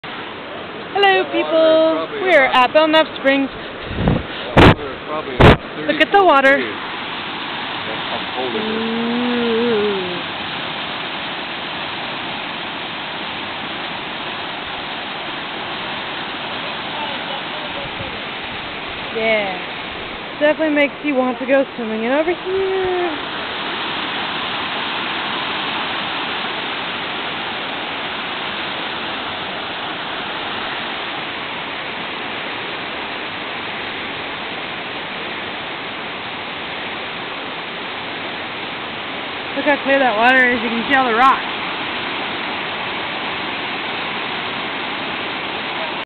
Jamie's One Day Road Trip Oregon Adventure Part 1
I went on an adventure! See some of the things I did at our first stop of the day. Belnap Hot springs with my mom and grandpa.